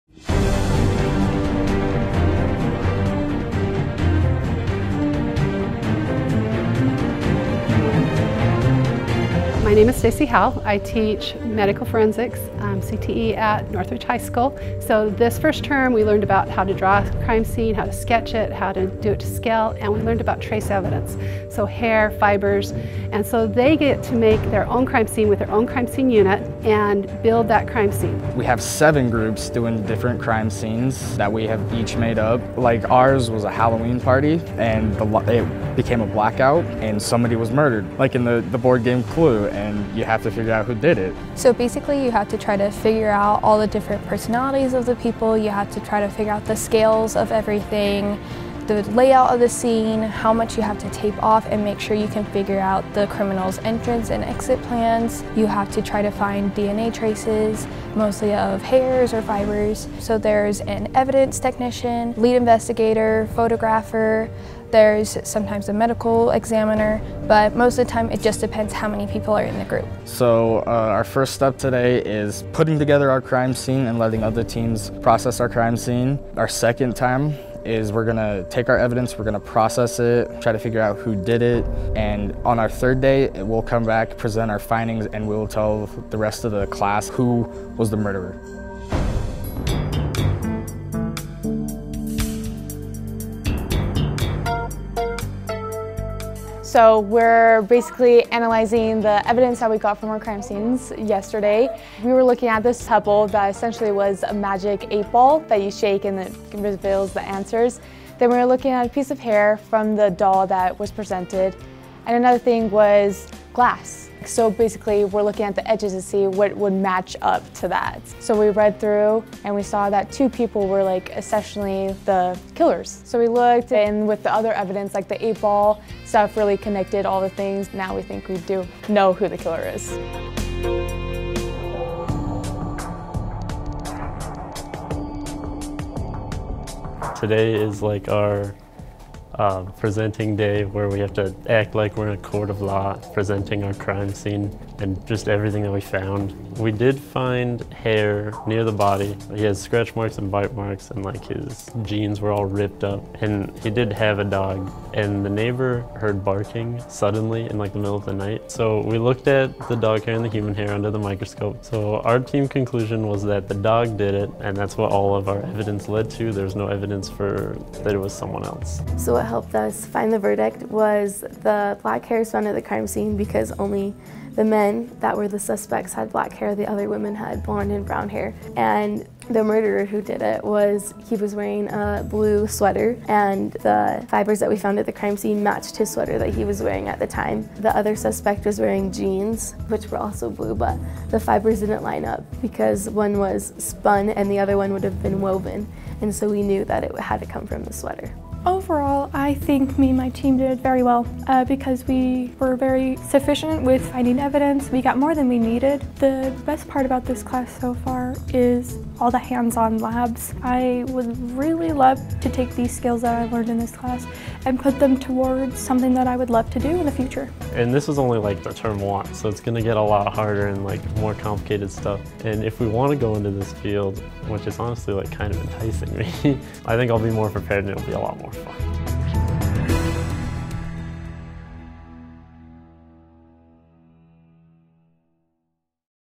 Forensics Project Northridge High
Students in Stacey Howell's Medical Forensics Course discover how to apply science in creating and solving a mystery.